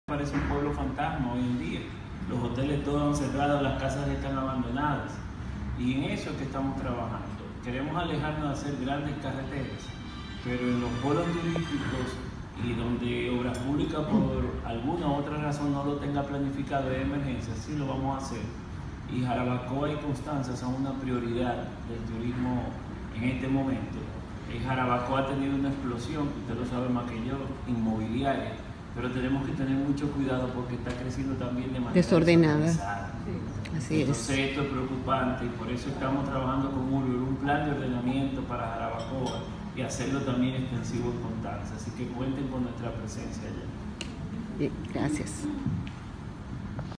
Ministerio de Turismo promoverá visitas a Constanza y Jarabacoa
El ministro de Turismo, David Collado, afirma que Constanza y Jarabacao son prioridad para el Ministerio de Turismo.